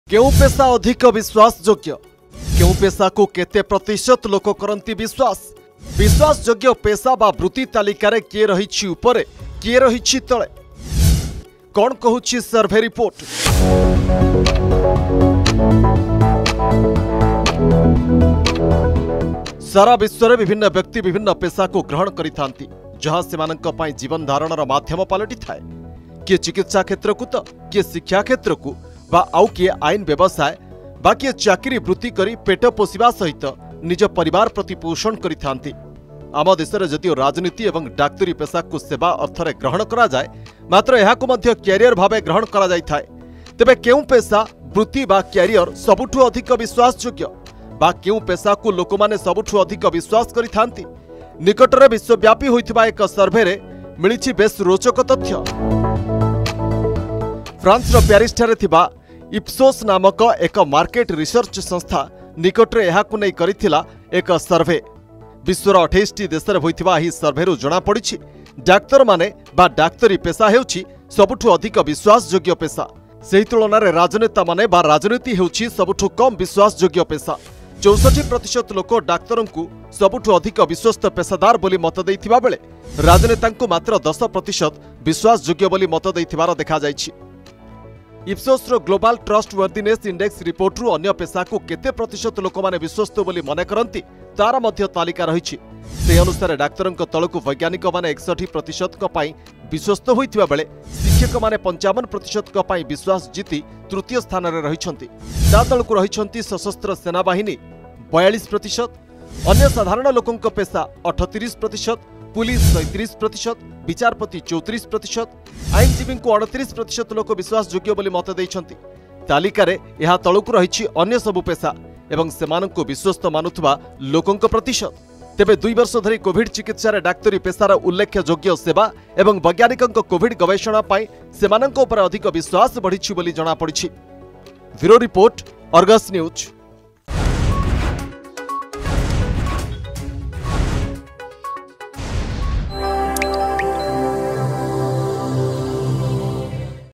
କେଉଁ ପେଶା ଅଧିକ ବିଶ୍ବାସଯୋଗ୍ୟ ?
କେଉଁ ପେଶା ଅଧିକ ବିଶ୍ବାସଯୋଗ୍ୟ ? କେଉଁ ପେଶାକୁ କେତେ ପ୍ରତିଶତ ଲୋକ କରନ୍ତି ବିଶ୍ବାସ ? ବିଶ୍ବାସଯୋଗ୍ୟ ପେଶା ବା ବୃତ୍ତି ତାଲିକାରେ କିଏ ରହିଛି ଉପରେ କିଏ ରହିଛି ତଳେ ? କ’ଣ କହୁଛି ସର୍ଭେ ରିପୋର୍ଟ ?